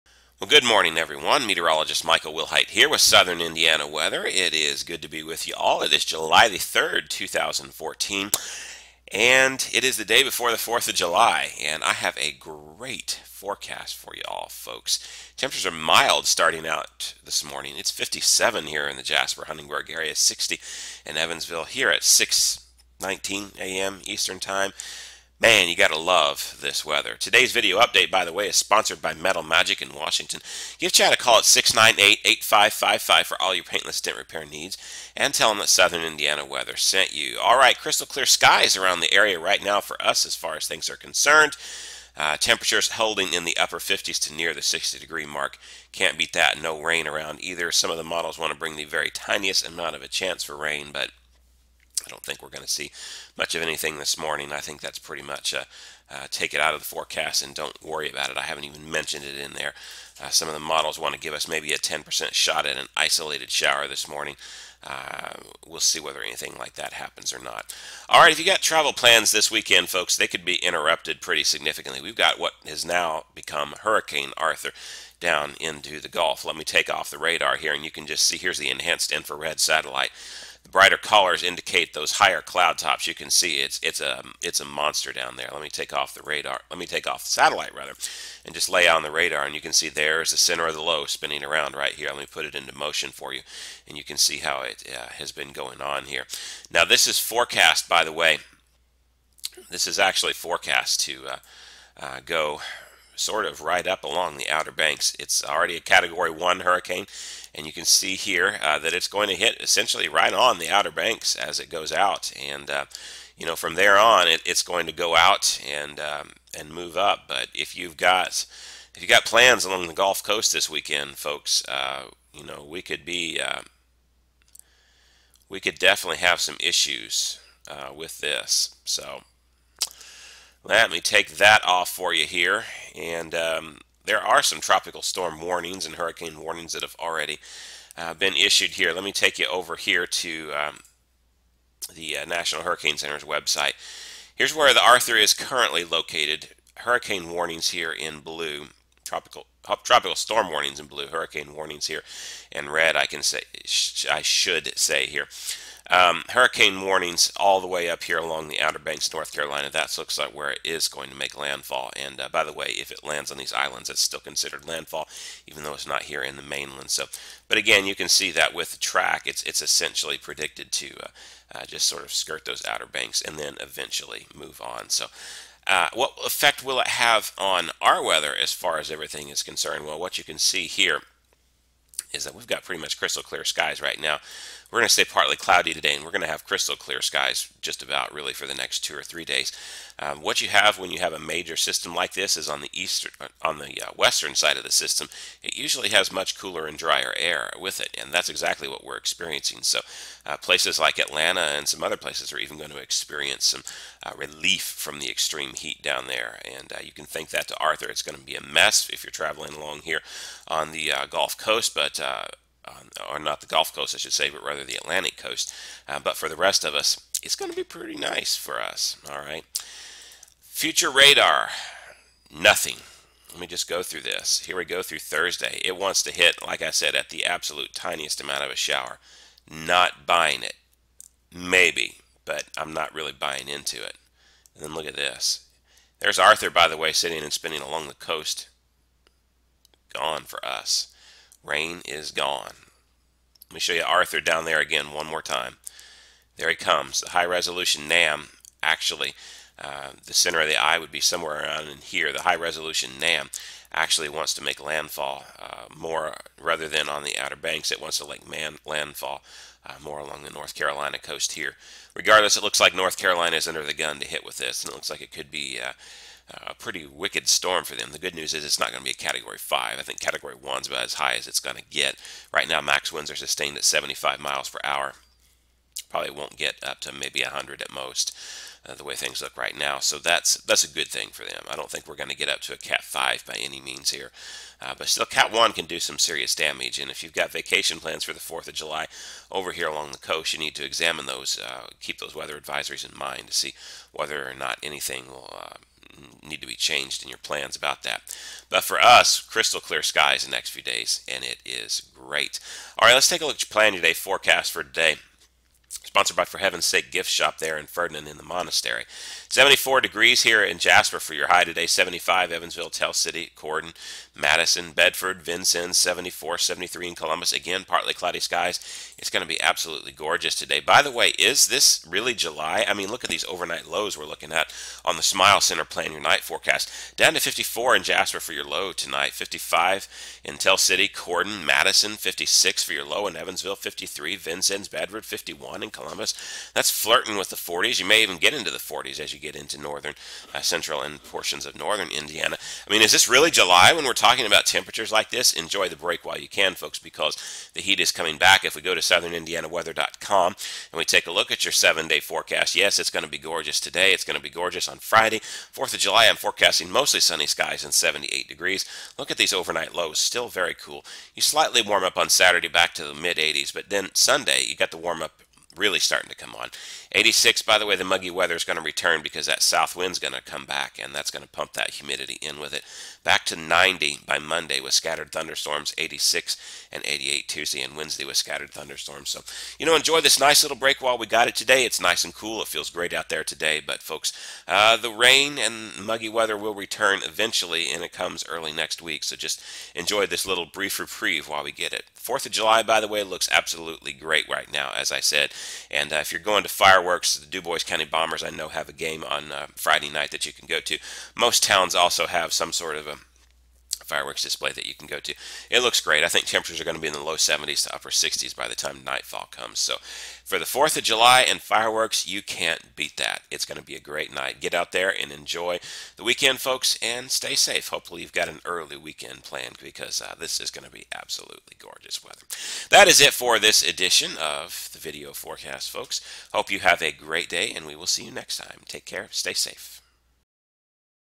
7-3-2014 Holiday Weekend Update
Meteorologist Michael Wilhite brings you a beautiful holiday weekend forecast, along with an update on Hurricane Arthur, which is poised to make landfall along the North Carolina coast Friday.